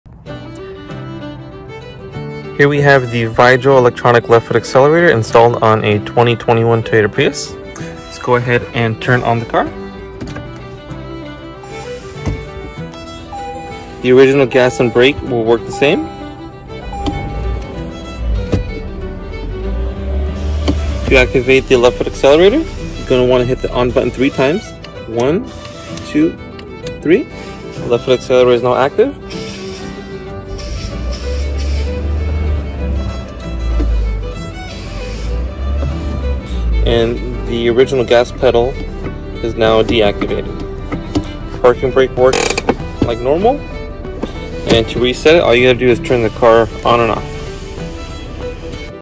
How It Works Veigel Electronic Left Foot Gas Pedal Installed on 2021 Toyota Prius
The Electronic Left Foot Accelerator allows a driver to operate an accelerator pedal on the left of the brake pedal with their left foot. The design mimics the original pedal for ease of use, comfort and retains full range of acceleration.

For maximized safety, the Electronic Left Foot Accelerator now requires the trained driver to activate the left pedal prior to each use to ensure that untrained drivers do not get access.

A push button switch is required to turn the system on and transfer function to the left accelerator pedal after the vehicle has been started. When done so, only the left pedal is functional and the (right) OEM pedal is non- operational. This safety feature also means that there is no need to add pedal guards.

The system always defaults to the right-side accelerator pedal after the vehicles ignition has been turned off.

We use only OEM proven circuits and OEM accelerator pedals to ensure the highest quality and reliability of the entire system.